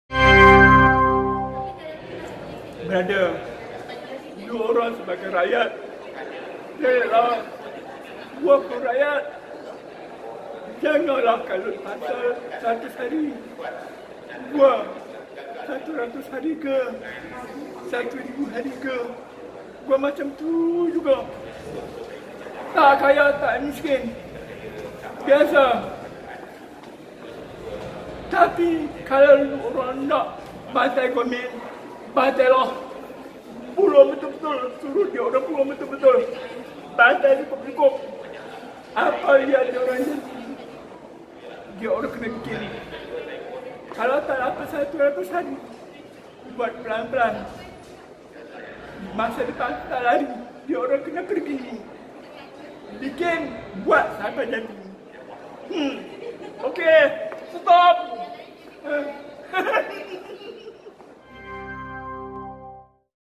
Puisi spontan Pyan Habib di lobi Parlimen, beri nasihat pada kerajaan, rakyat
Apa dia orang janji mereka kena bikin. Itu antara bait-bait puisi Pyan Habib.